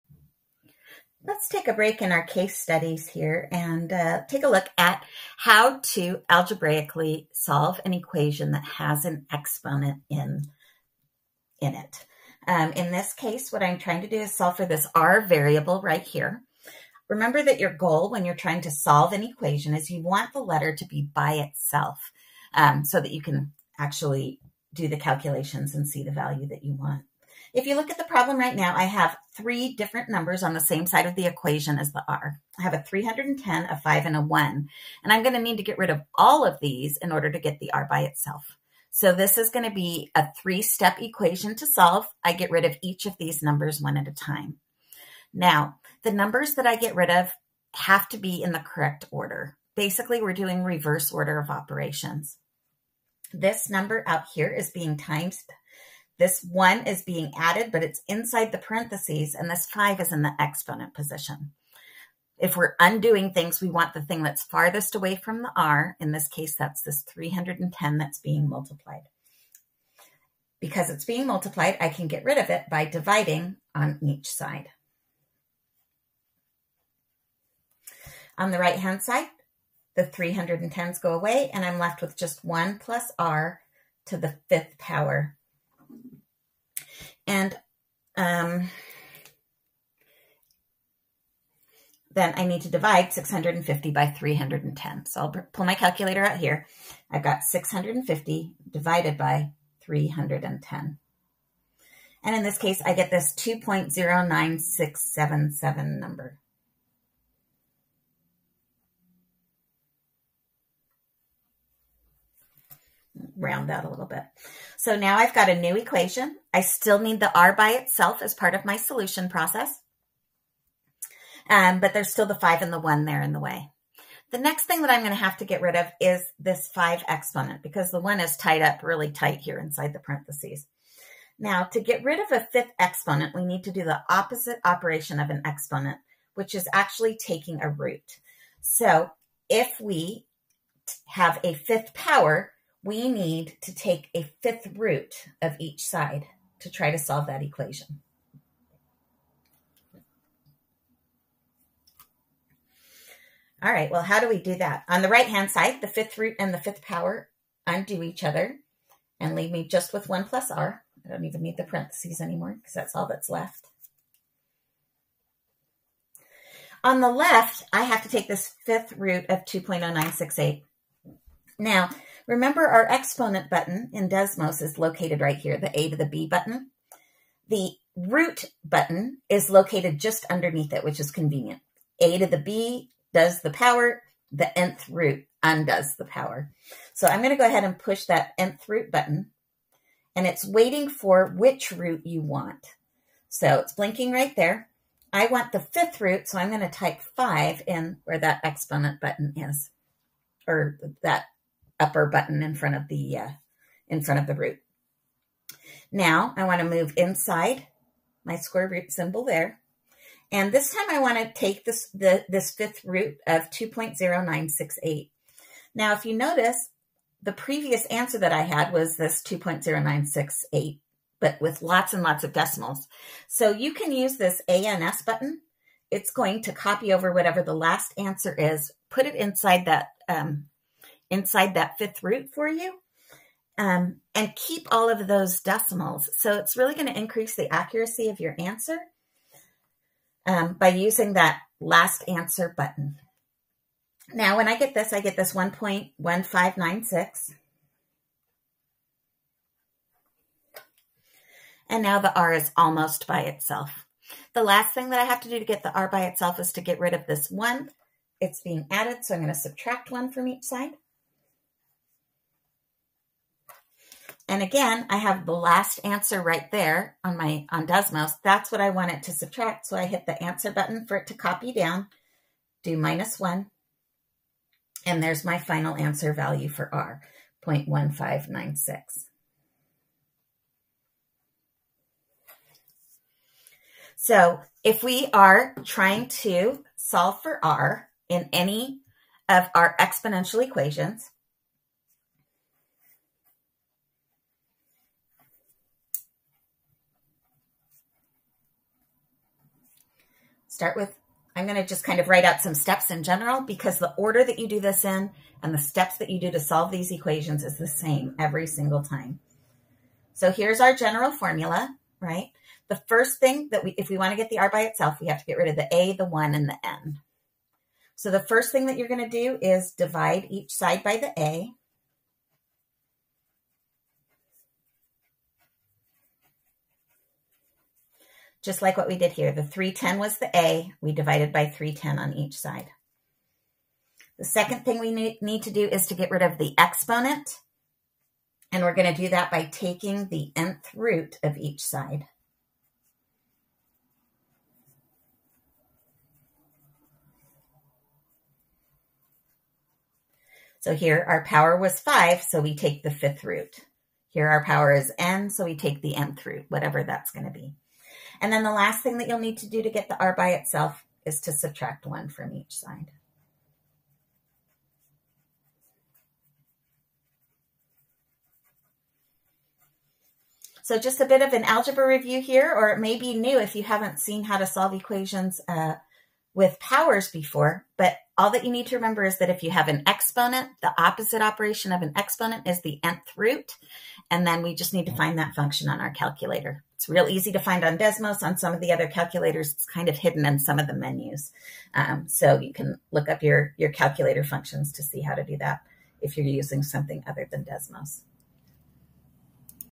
The Algebra Behind Solving an Exponential Equation for "r"
Math in Society lesson reviewing the algebra required to solve an exponential equation when the unknown is being taken to a power.  Math skills include solving exponential equations and using a calculator(Desmos) to find nth roots.